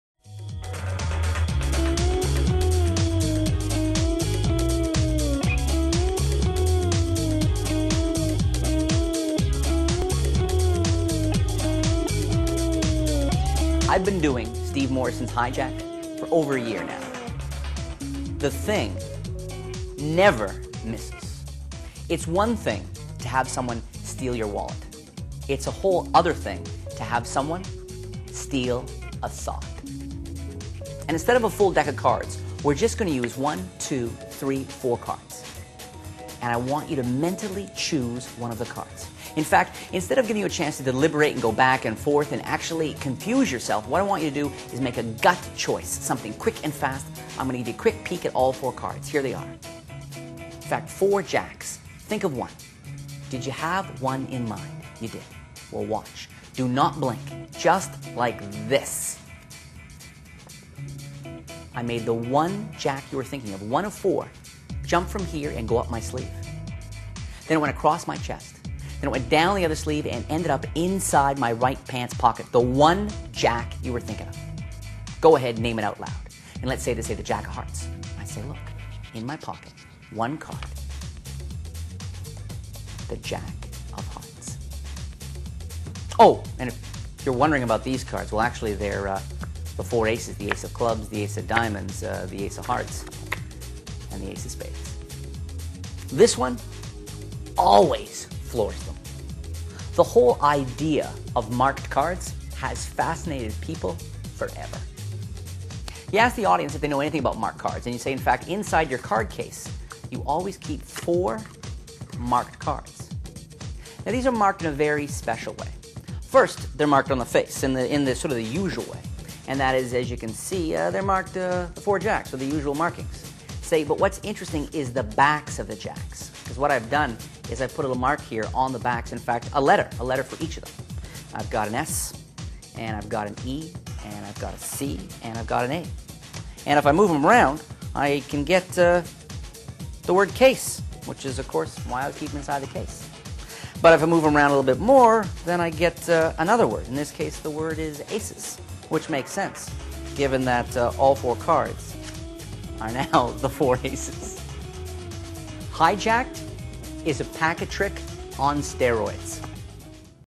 Chinese Coin (CH0017) Black & Yellow by Tango Magic - magictricks.co.uk
- High quality imitation Chinese coins, perfect for all of your routines. Highly-polished finish with raised Chinese characters on both sides. These are NOT cheaply stamped and painted washers - they're custom-made coins, crafted with the magician in mind.